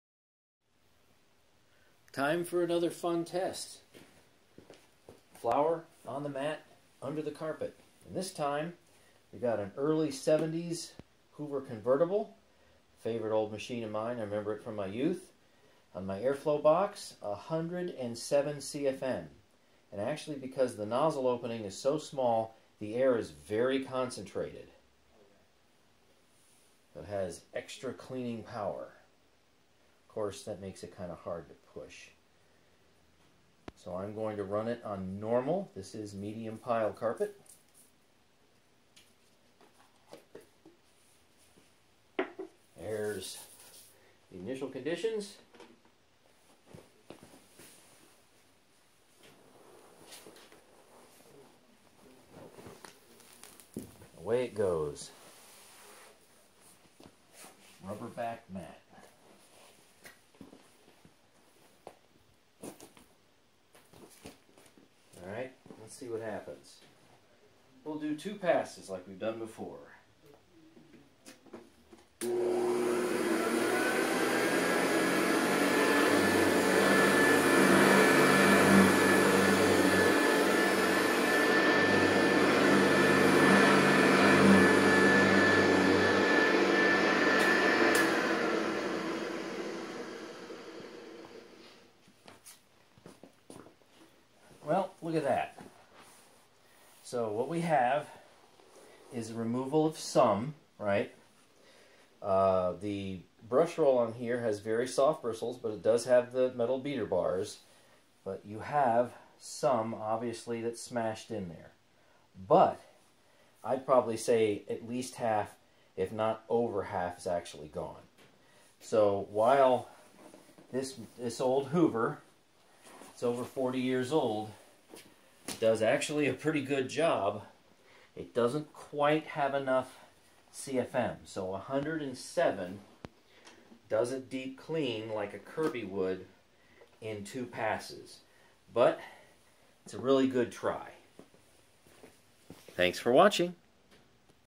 Hoover Convertible U4007 Mat Under the Carpet Test
How will this 40+ year old golden oldie perform?